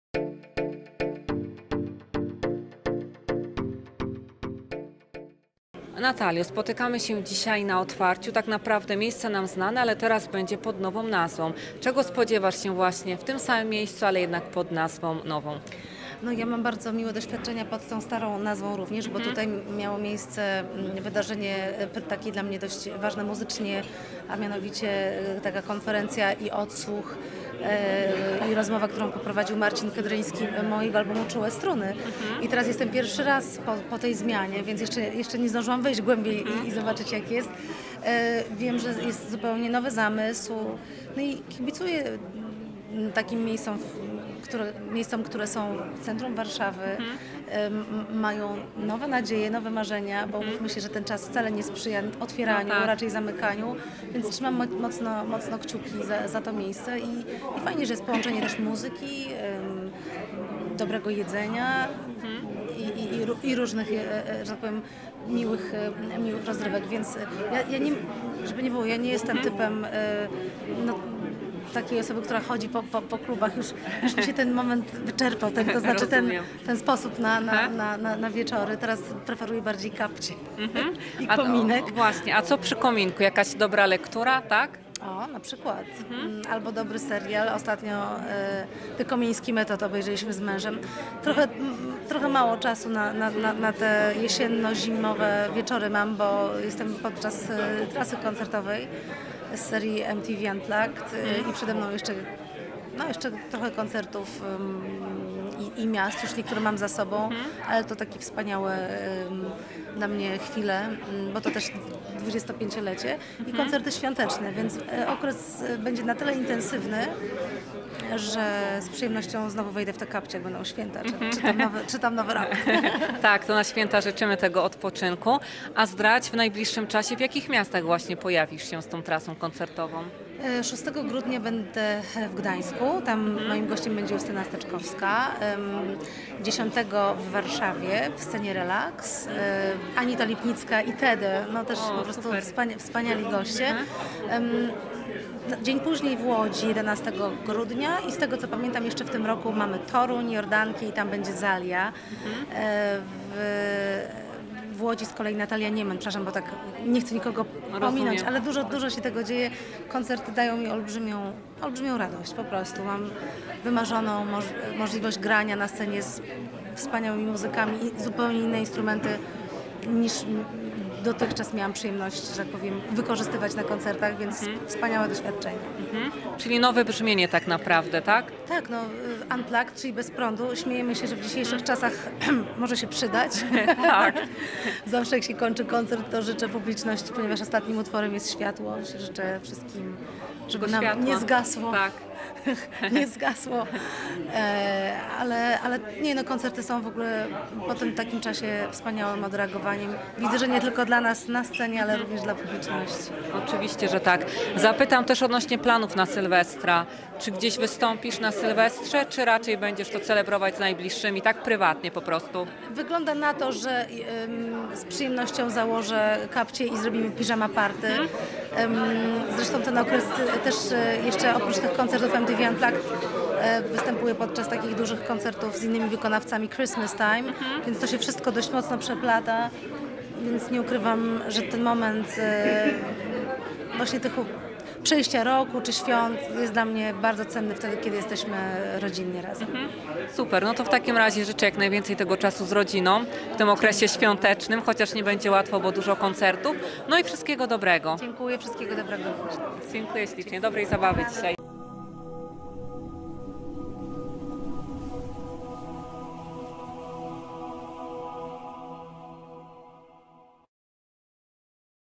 Natalia Kukulska o nowo otwartym klubie CHATON w Wa-wie
Natalia Kukulska o nowo otwartym klubie CHATON  w Warszawie oraz o udziale Karoliny w nowej produkcji filmowej, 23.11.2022 Warszawa

Dorbur Amber | Amber jewellery - partnerem honorowym Telewizji ATV

Reporter: Anrika Koperska
Operator kamery: Andrzej Tarkowski-Kiliszewski
Montaż: Andrzej Tarkowski-Kiliszewski
Kierownik planu zdjęciowego: Andrzej Tarkowski-Kiliszewski
Studio filmowe:  ATV FILM STUDIO AND NEWSDAY PRESS AGENCY
Produkcja: Telewizja ATV 2022